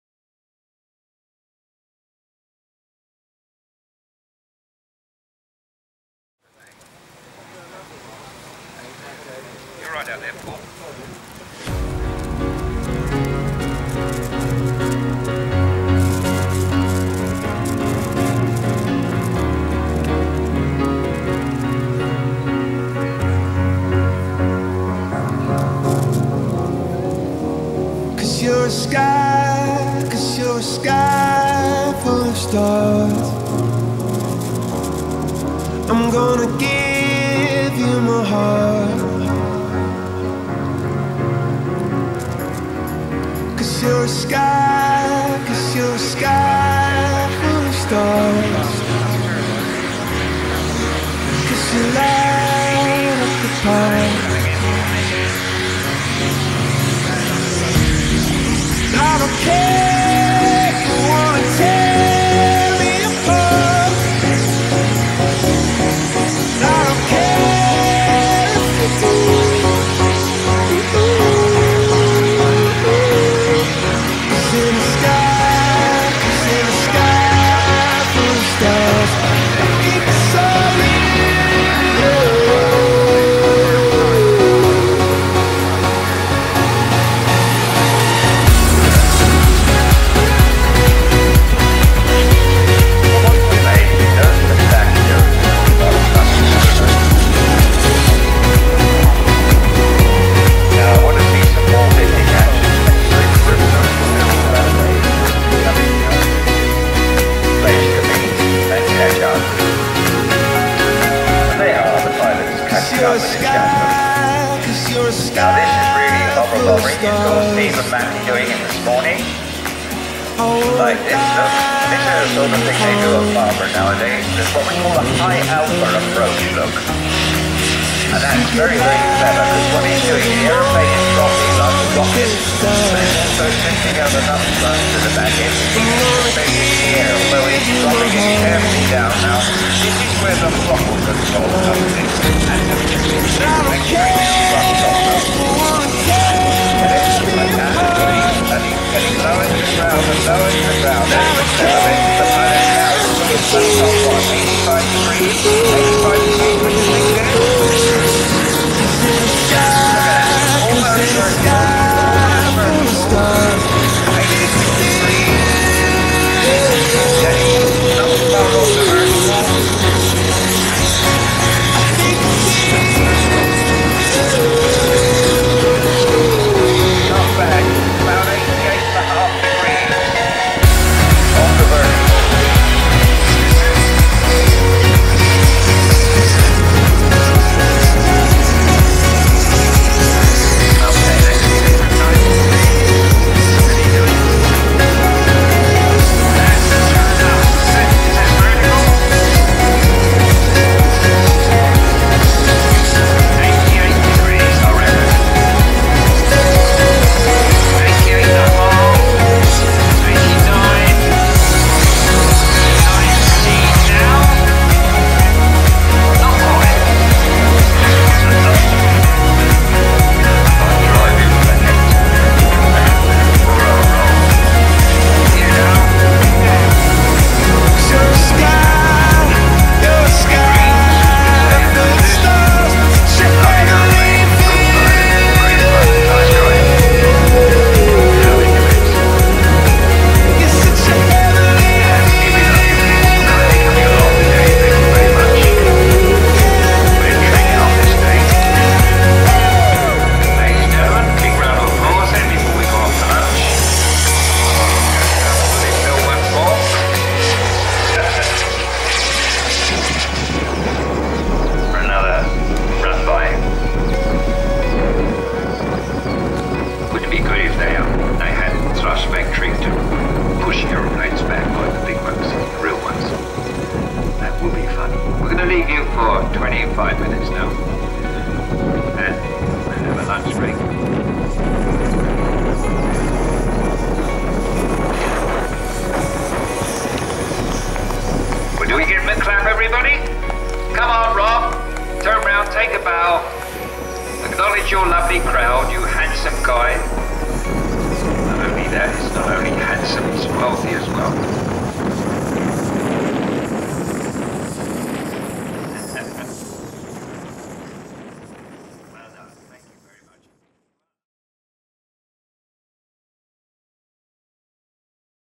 Long Marston Air Show ( sunday 2015 )
Long Marston Air show 2015, filmed on the Sunday. it was very cold for late may and only a few pilots braved the extreme weather !